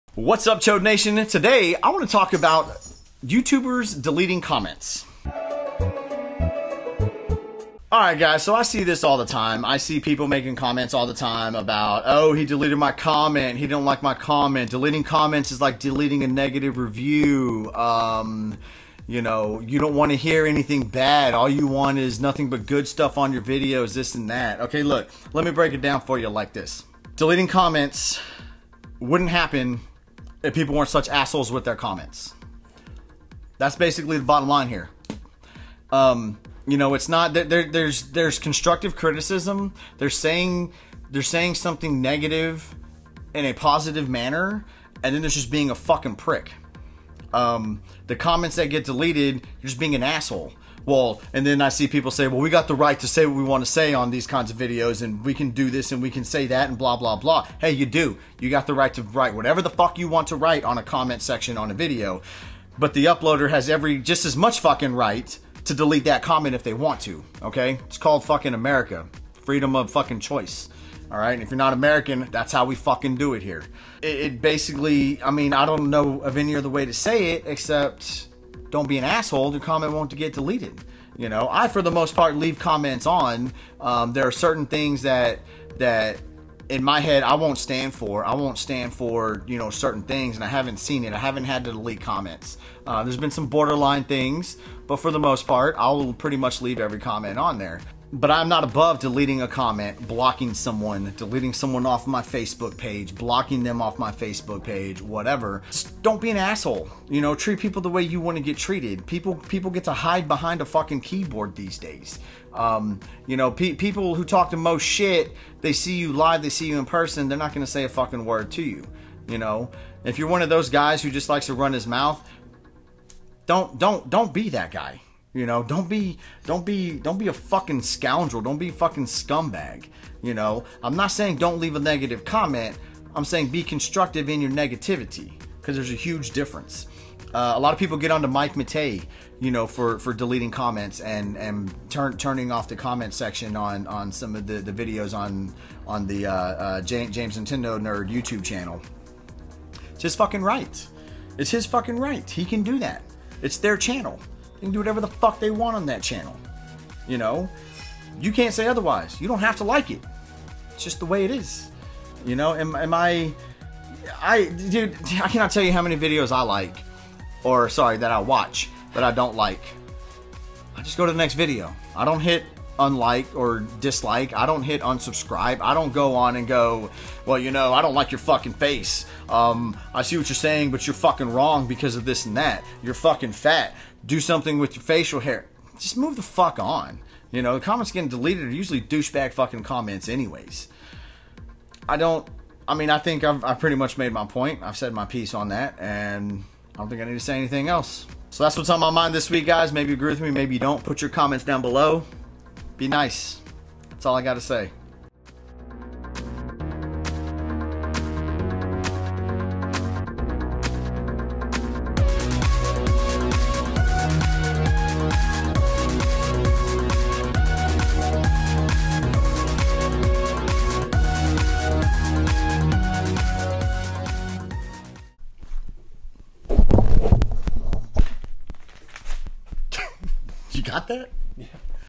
Youtubers Deleting Comments - What's on Jay's Mind
Today Jay talks about deleting comments in the comment section and having the right to do so as a content creator.